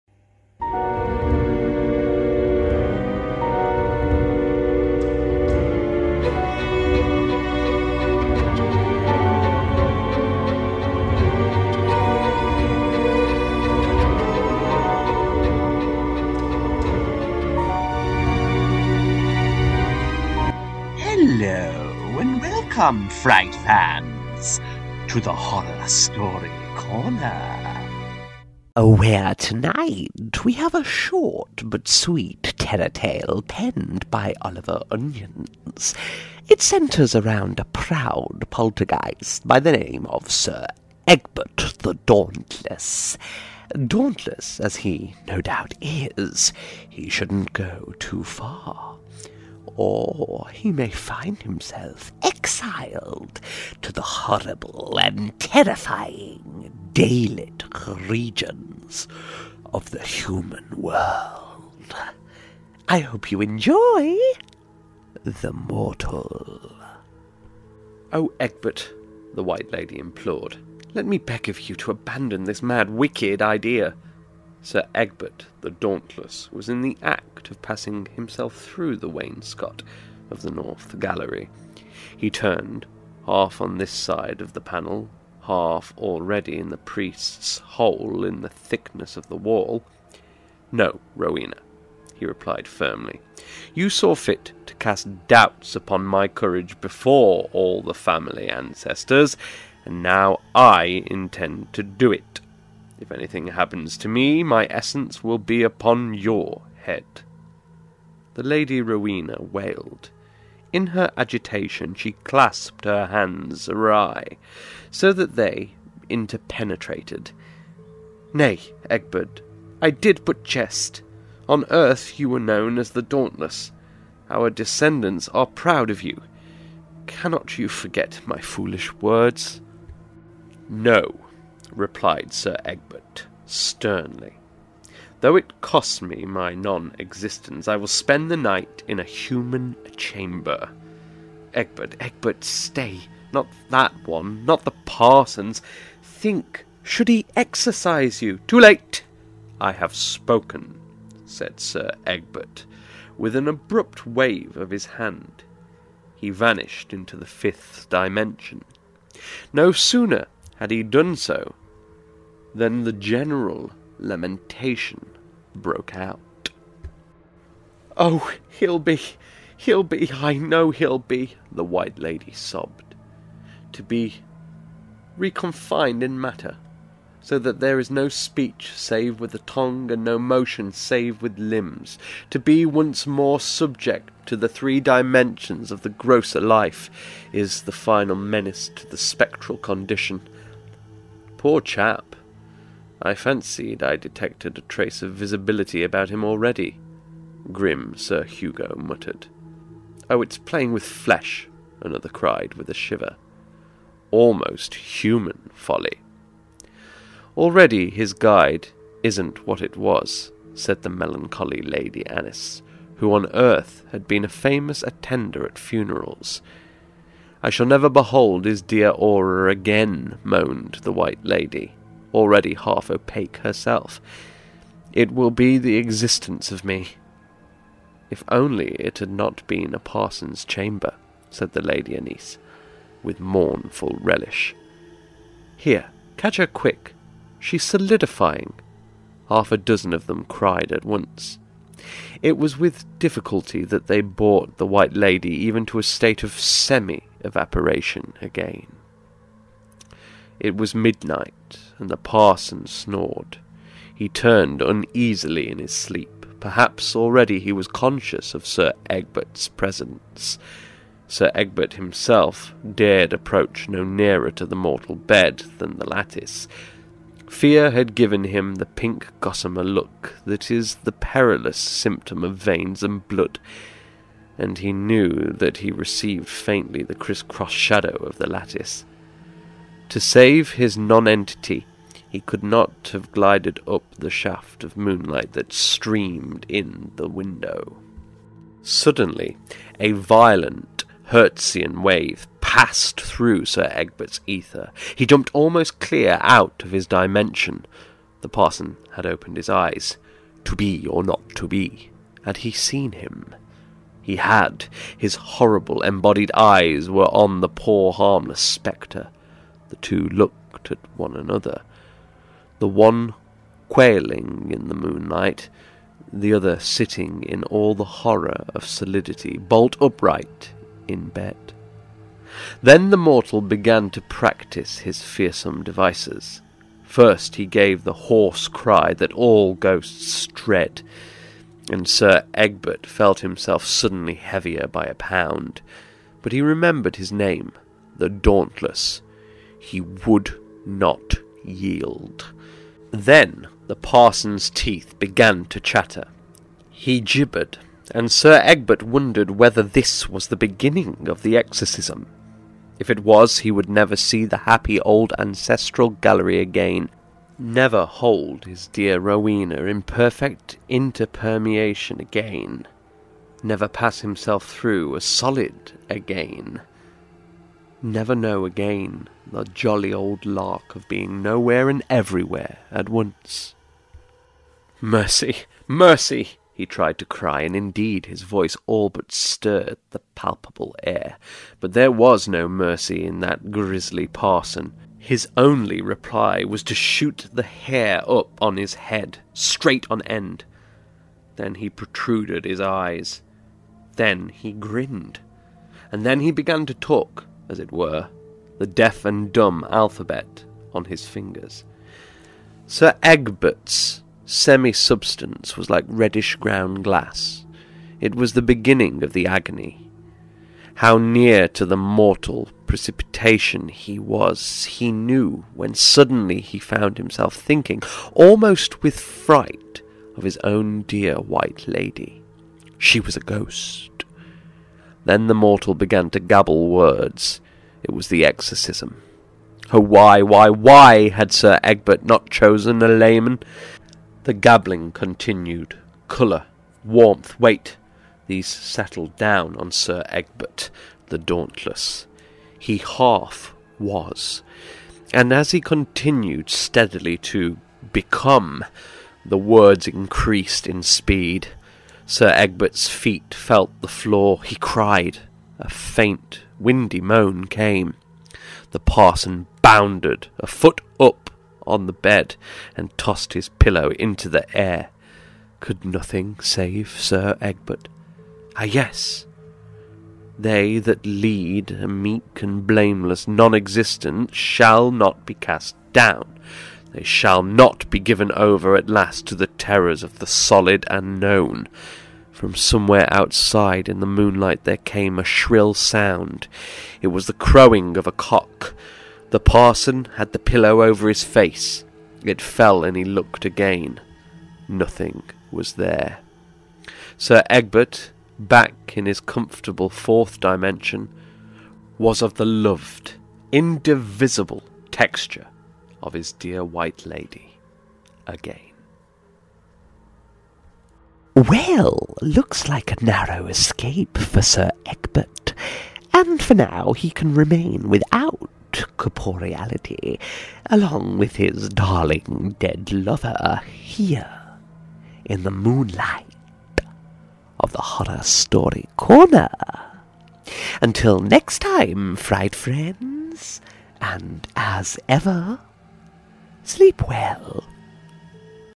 The Mortal by Oliver Onions [A short Ghost Story] Read by The Horror Story Corner
Hello and welcome fright fans to this, The Horror Story Corner. Where tonight we have a short but sweet terror tale penned by Oliver Onions. It centres around a proud poltergeist by the name of St Egbert the dauntless. Dauntless as he no doubt is, he shouldn’t go too far, or he may find himself exiled to the horrible and terrifying daylit regions of the human world.

I hope you enjoy, The Mortal…